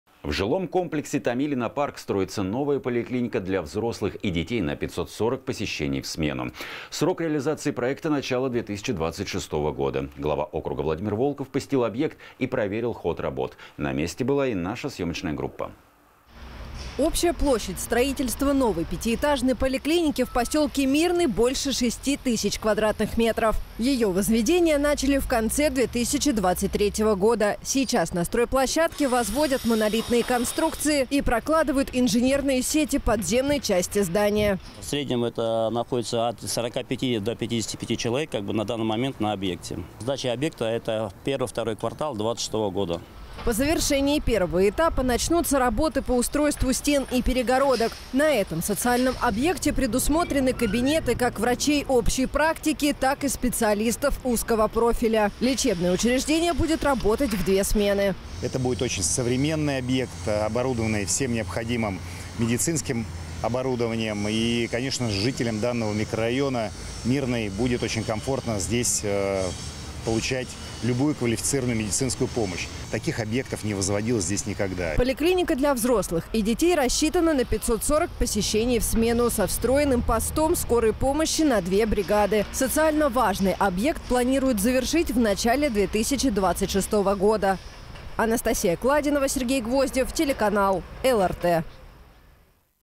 В поселке Мирный строится новая поликлиника
В жилом комплексе «Томилино Парк» строится новая поликлиника для взрослых и детей на 540 посещений в смену. Срок реализации проекта начало 2026 года. Глава округа Владимир Волков  посетил объект  и проверил ход работ. На месте была и наша съемочная группа.

Общая площадь строительства новой пятиэтажной поликлиники в поселке Мирный больше шести тысяч квадратных метров.  Ее возведение начали в конце 2023 года. Сейчас на стройплощадке возводят монолитные конструкции и прокладывают инженерные сети подземной части здания.

Николай Кривонос, заместитель директора строительной компании: «В среднем здесь находятся от 45 до 55 человек на данный момент на объекте. Сдача объекта – это первый – второй квартал 2026 года».

По завершении первого этапа, начнутся работы по устройству стен и перегородок.  На этом социальном объекте предусмотрены кабинеты как врачей общей практики, так и специалистов узкого профиля. Лечебное учреждение будет работать в две смены.

Владимир Волков, глава г.о. Люберцы: «Это будет очень современный объект, оборудованный всем необходимым медицинским оборудованием. И, конечно же, жителям данного микрорайона Мирный будет очень комфортно здесь получать любую квалифицированную медицинскую помощь.  Таких объектов здесь никогда».

Поликлиника для взрослых и детей рассчитана на 540 посещений в смену со встроенным постом скорой помощи на 2 бригады. Социально-важный объект планируют завершить в начале 2026 года.